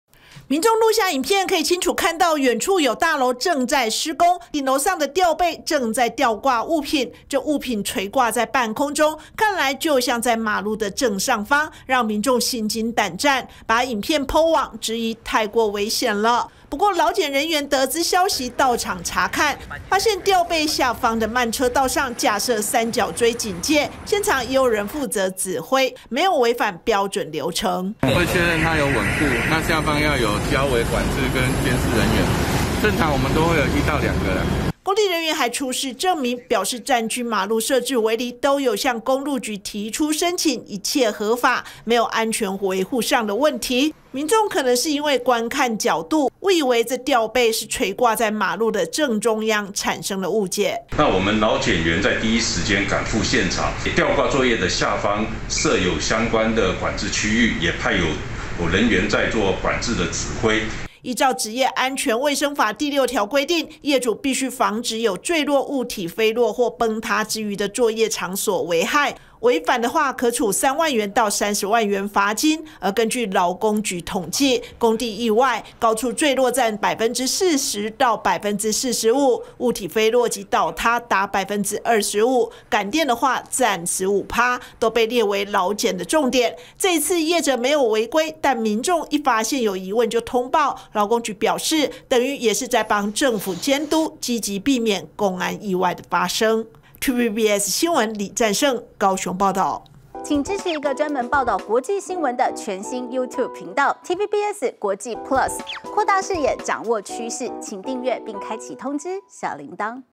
民眾心驚!大樓吊掛重物垂馬路上空?勞工局:符合規定莫慌｜TVBS新聞
高雄市明誠四路上的大樓建案，被民眾拍攝影片po網，質疑吊臂在半空中吊掛物品，直接懸在馬路的正上方，十分危險！不過，勞檢人員在第一時間趕赴現場，確認慢車道上，有以三角錐架起警戒區域，也有人員現場管制，完全符合規範，可能是觀看角度讓民眾誤解了！